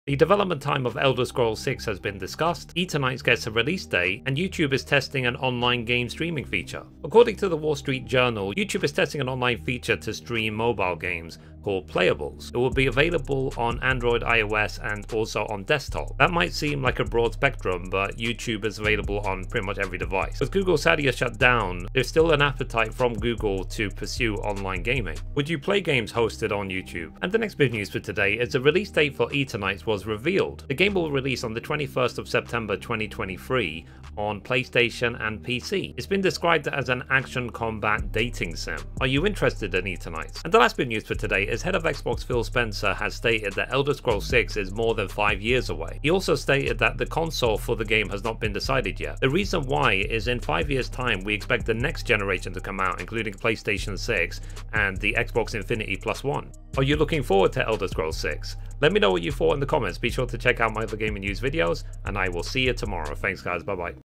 Elder Scrolls 6 Development Time
The Head of Xbox has discussed the development time for Elder Scrolls 6. I also discuss the release date for Eternights, and the new game streaming platform from YouTube called Playables.

Timestamps:
00:00 Intro
00:06 YouTube Playables Leaked
00:26 Eternights Release Date
00:40 Elder Scrolls 6 Development Time

Mithrie